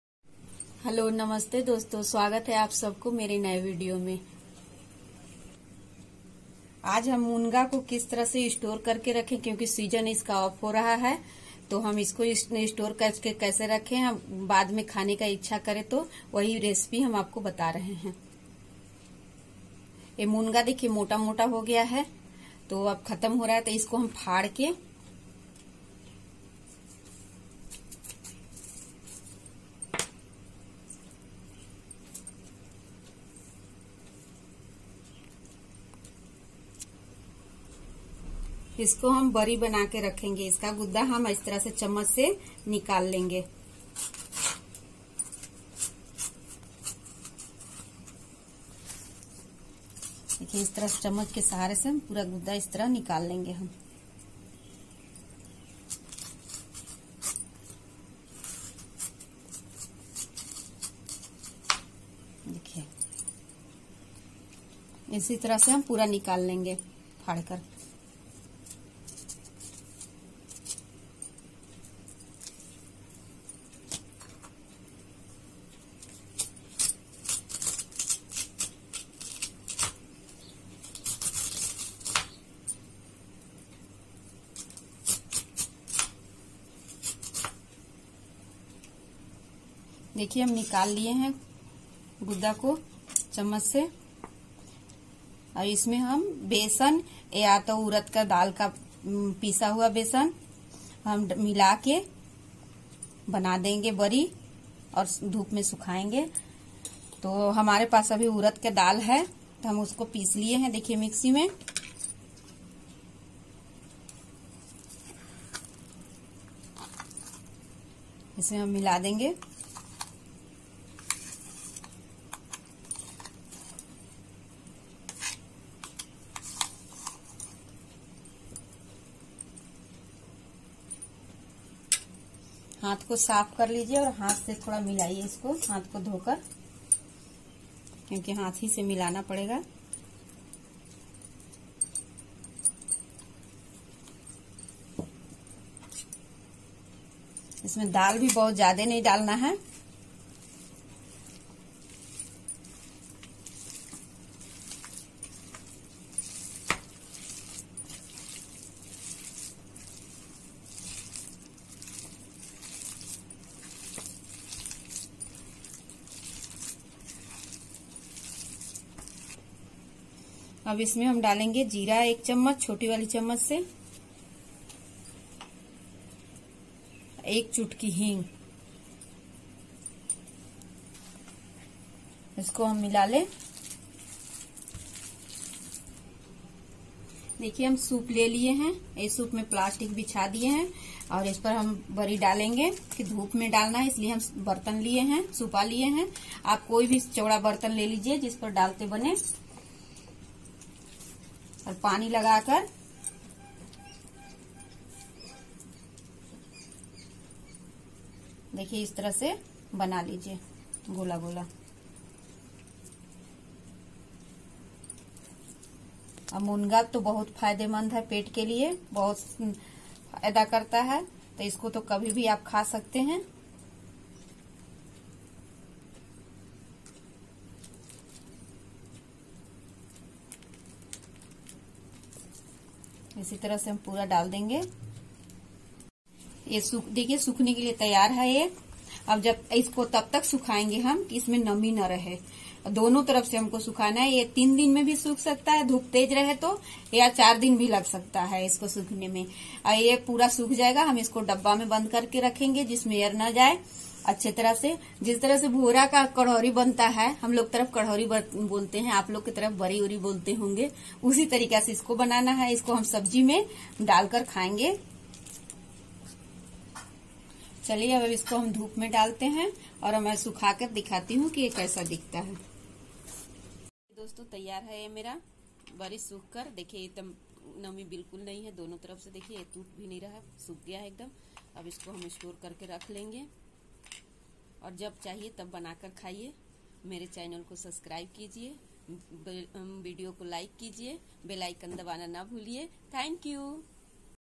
How to store drumstick(munga) for long time.
Hi friends,
shared a way to store drumsticks for long.